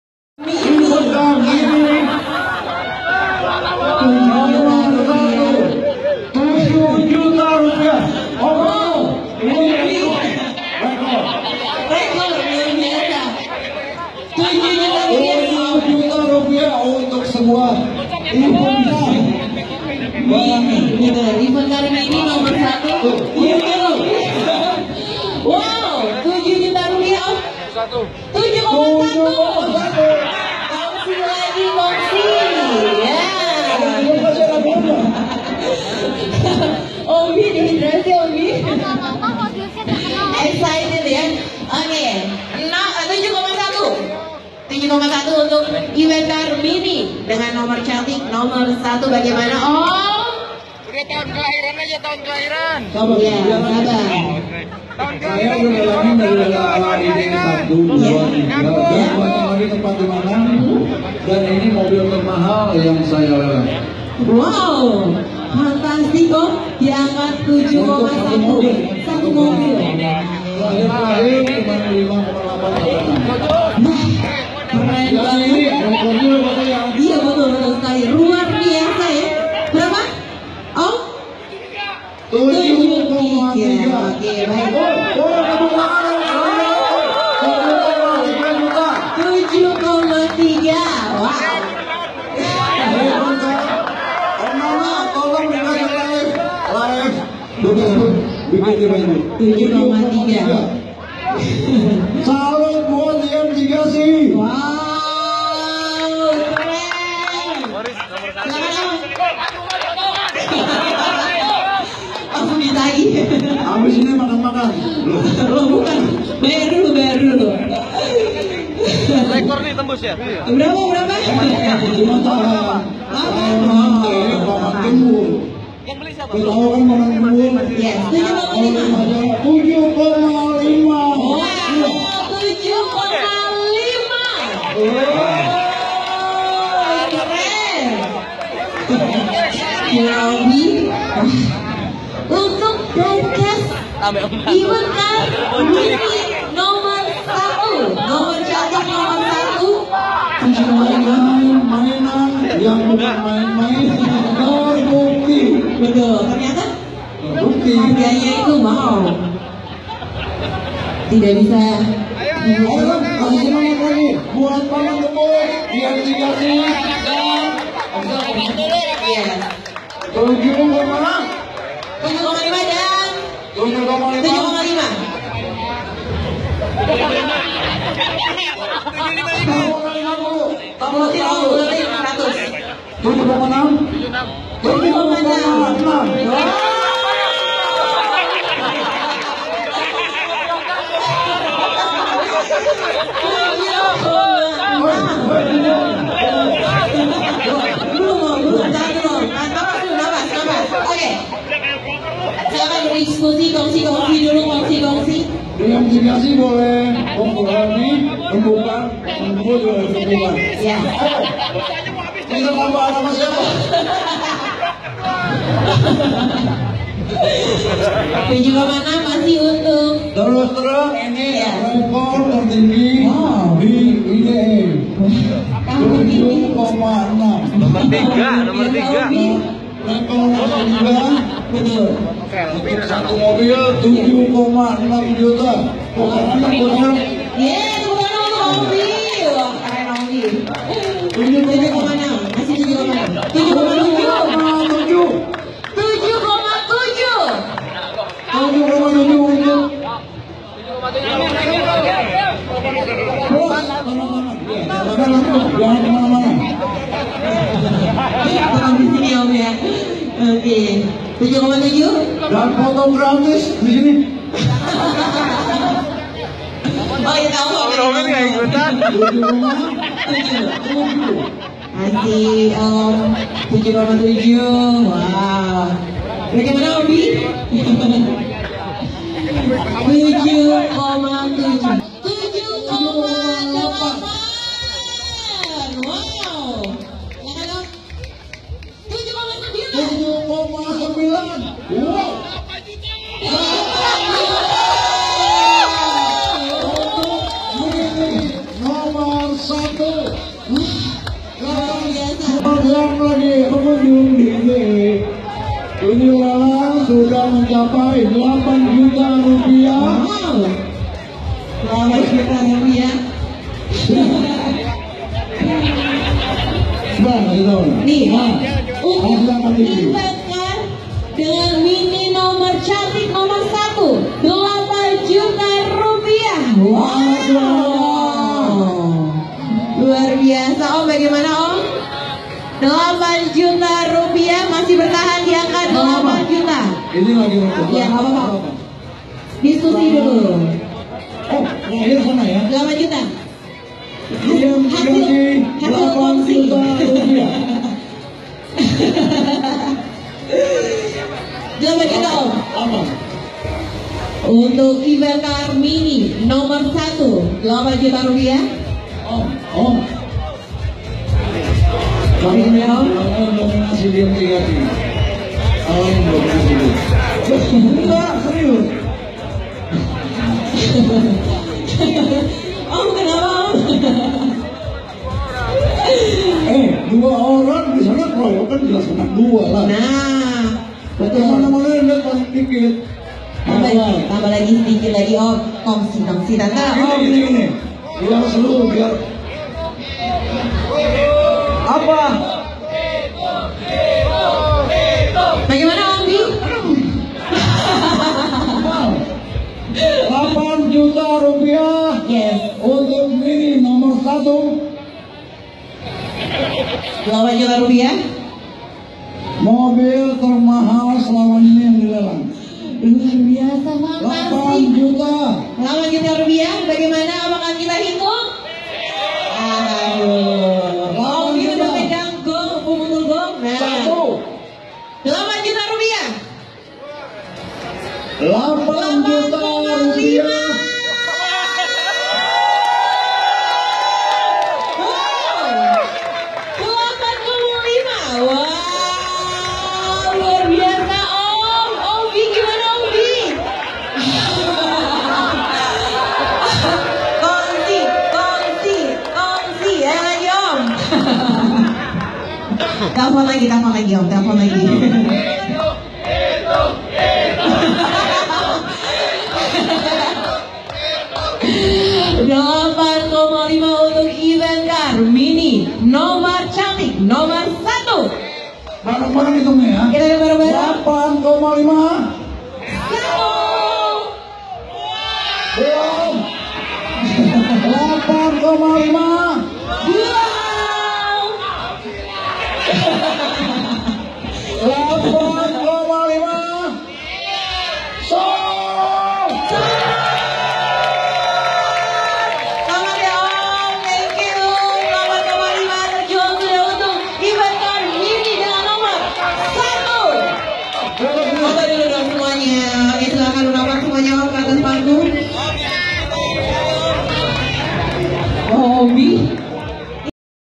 WOW! Harga Mobil-mobilan Tembus 8,5 juta!!! Hot Wheels Mini Morris Indonesia Diecast Expo 2016
Lelang Event Car Indonesia Diecast Expo 2016.
Hot Wheels Mini Morris 1 of 50.
Limited Edition.
Pemenang Lelang Om Henry Haliem DeLuckz dengan harga Rp. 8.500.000,-

Video dari Om Nano Rokston